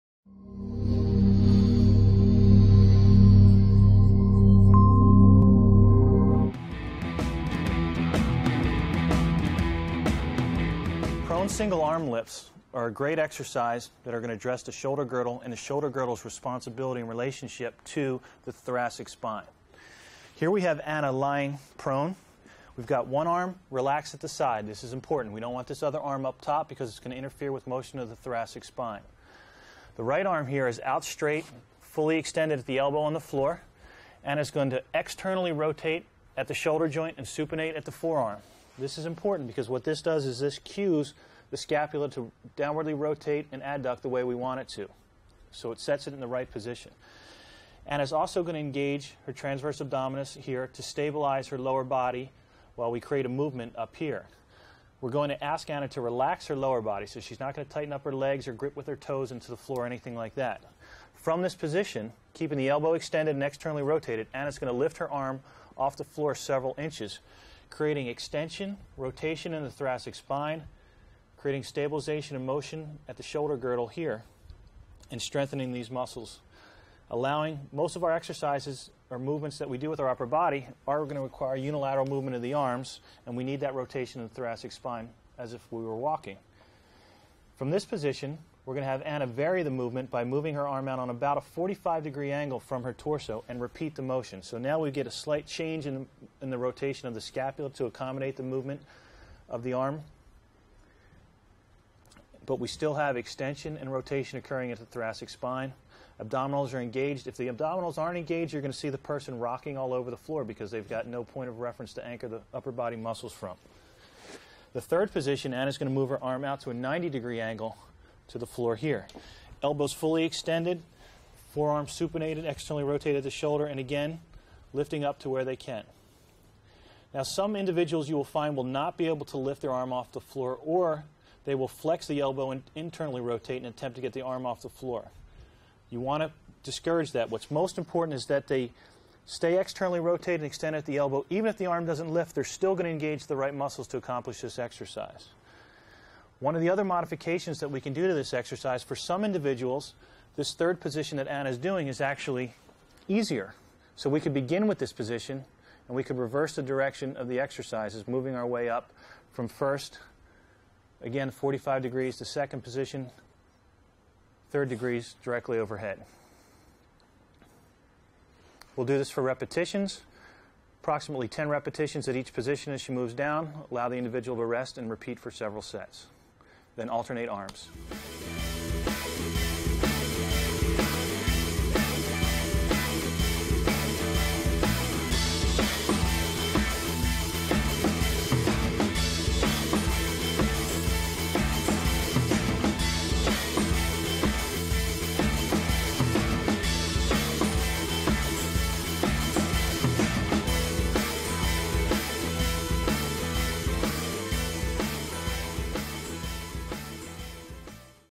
Prone Single Arm Lifts
This corrective exercise for the shoulder and thoracic spine is significantly more challenging then it appears.  A great exercise to cue the thoracic spine and scapula to better "set" their anchor points for more controlled and stable motion of the shoulder.